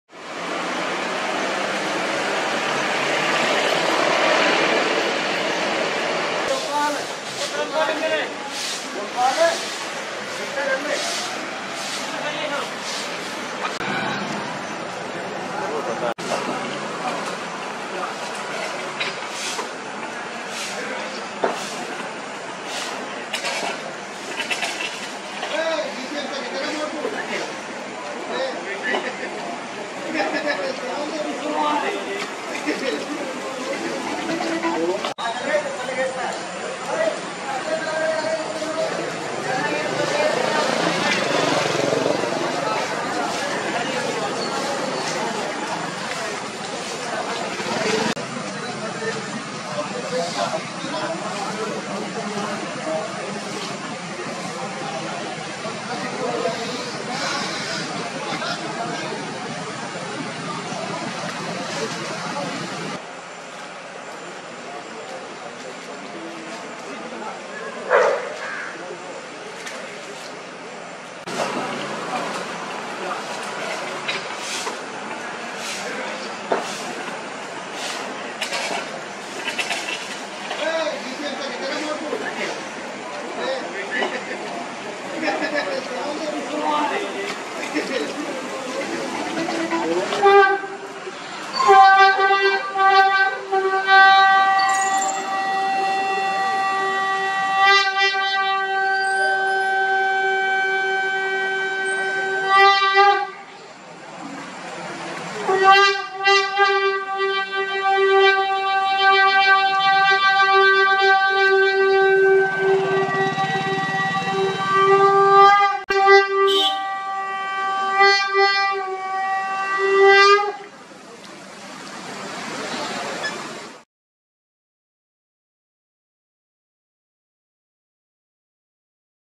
Parshurameshwar Mahadev Mandir - Devotees perform rituals outside temple gates due to Covid Pandemic
The entrance to the Parshurameshwar Mahadev Mandir on the Baghpat-Meerut highway is sealed as a preventive measure against the spread of Covid 19. Still this does not stop devotees from offering prayers and performing rituals outside the temple gates. A donation box is kept outside for the devotees to offer what they wish to. A small family of  Rhesus Macaque is sitting on the temple walls. A devotee is blowing a conch shell while policemen are on duty next to the temple gates to check violation of protocols.